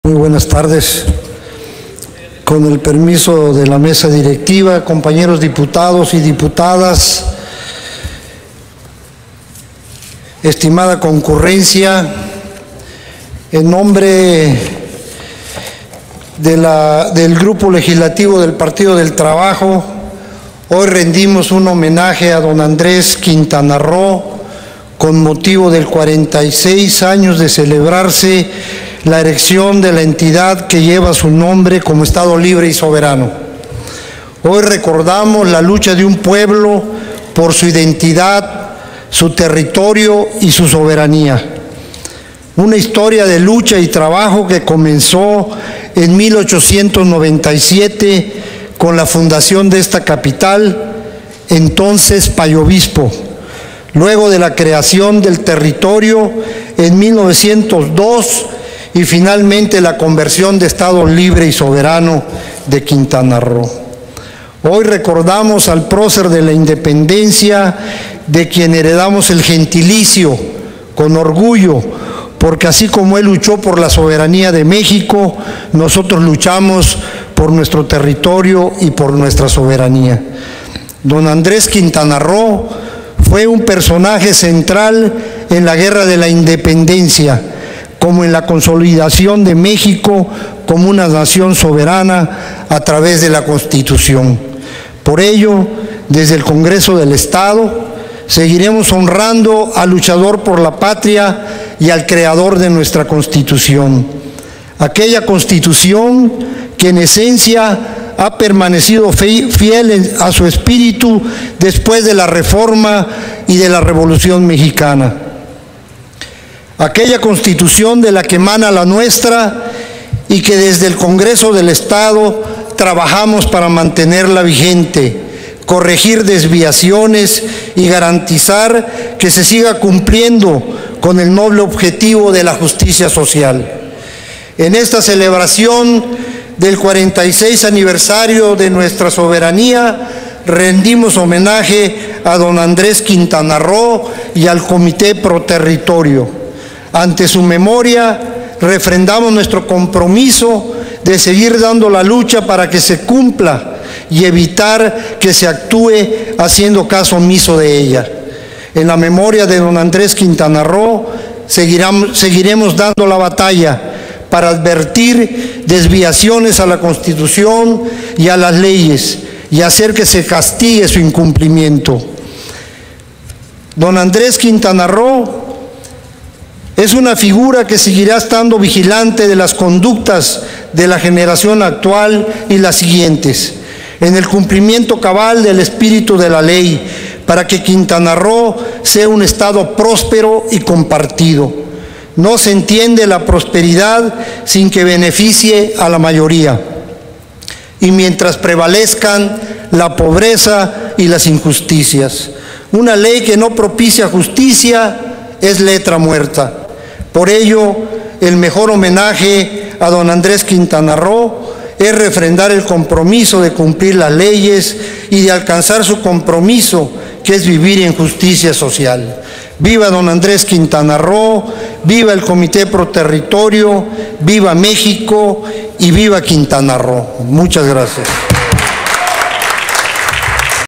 Intervención del Dip. Roberto Erales Jiménez. PT (9/Oct/2020)
Intervención del Diputado Roberto Erales Jiménez, del PT, en relación al 46 Aniversario del Estado libre y soberano de Quintana Roo